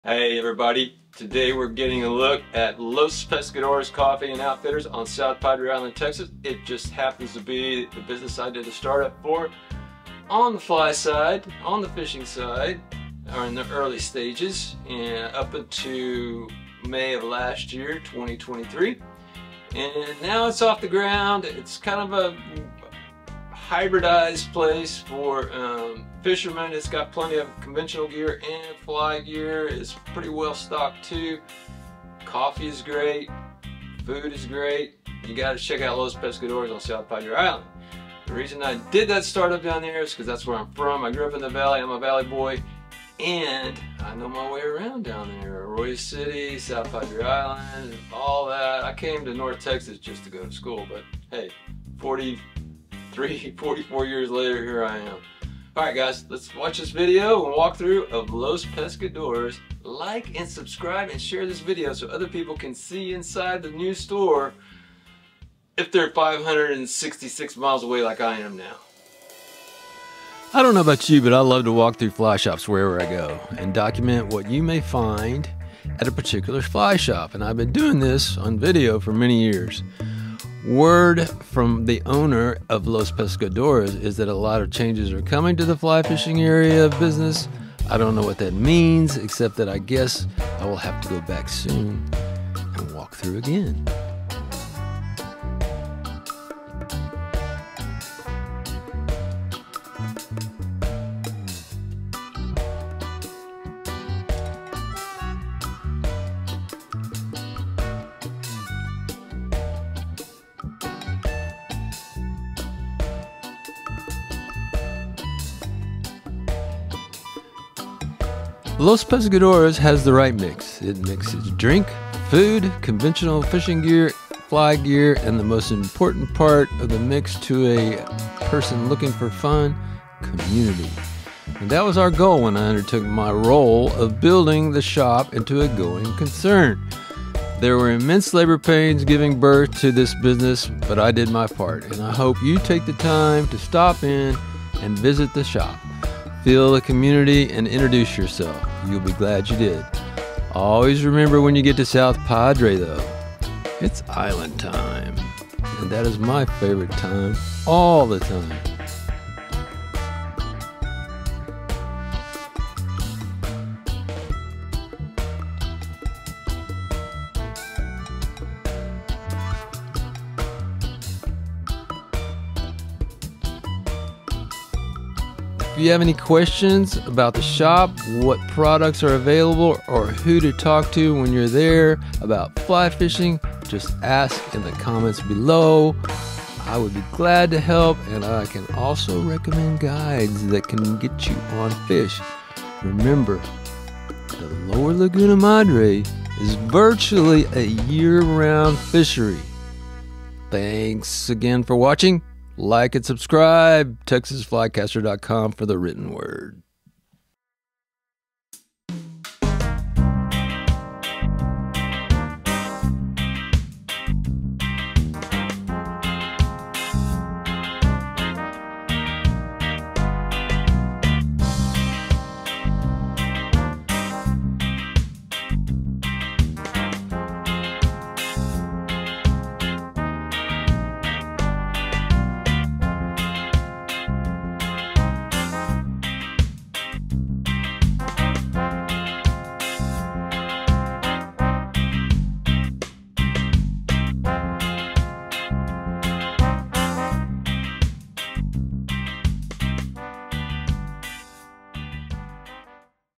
Los Pescadores Coffee and Fly Shop South Padre Island Texas - A Walkthrough of the New Shop
Walk with me through Los Pescadores Coffee and Outfitters on South Padre Island, Texas, is the most recently added fly shop to the list of Texas fly shops, and believe me there aren’t that many of them around! My concept for the Los Pescadores startup was to stick to a "Destination" fly shop that caters to fishers and fly fishers COMING TO South Padre Island, and believe me, there are thousands of you coming to the Island ever year and almost all year long! Keep in mind, the next closest real fly shop is 132-miles away. Talk about controlling the market? Los Pes is set for success.

You may recall that I worked directly on the startup phase of Los Pescadores, from the fly fishing product lines to naming some of the coffee blends you find behind the counter. It is great to see it open, and from here their future looks pretty bright, and I don’t think that’s the Valley sun getting into my head!

I am always open to consulting on fly related shops. I learned plenty going through the pregnancy of this business, and think the baby seems to be off to a good start.

If you are in need of MORE INFORMATION about Los Pescadores, their products they have there for purchase, or finding a good guide who knows fly fishing?

I love to walk through fly shops wherever I go, and document what you may find at a particular fly shop, and I have been doing this for years. Word from the owner of Los Pescadores is that a lot of changes are coming to the fly fishing area of the business. I don’t know what that means, except I guess I will have to go back soon and walk through again!

Los Pescadores has the right mix. It mixes drink, food, conventional fishing gear, fly gear and the most important part of the mix - community. And that was our goal when I undertook my role of building the shop into a going concern. There were immense labor pains giving birth to this business, but I did my part, and I hope you take the time to stop in and visit the shop, feel the community, and introduce yourself. You will be glad you did.

Always remember - when you get to South Padre? It’s Island time. And that is my favorite time all the time. And be sure to check out their Saturday fly tying sessions!

If you have any questions about the shop, what products are available or who to talk to when you’re there - about fly fishing - just ask in the comments below! I would be glad to help, and I can also recommend guides that can get you on fish. Remember - the Lower Laguna Madre is virtually a year round fishery!